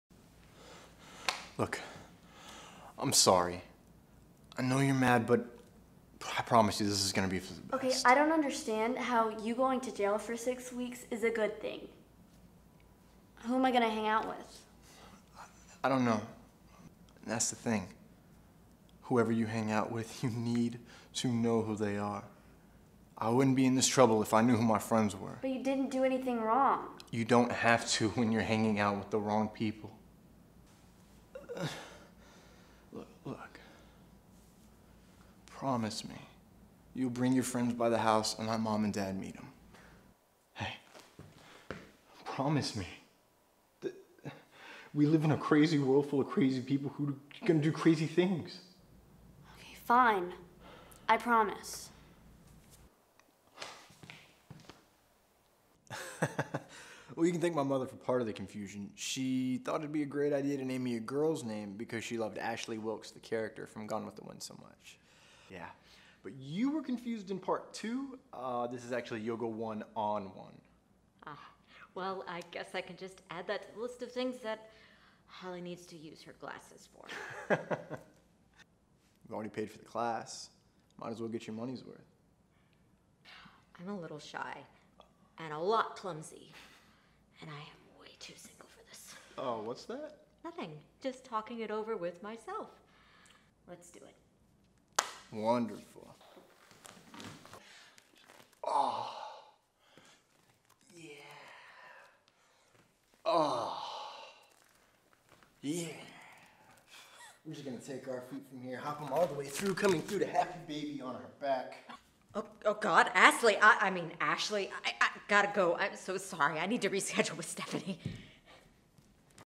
Thomas Carter Rochester | Acting Reel
1 Dramatic and 1 comedic combination reel.

Written by Teance Blackburn
Directed by Darla Briganti
Shot by Kevin Almodovar
Produced by Ann Marie Crouch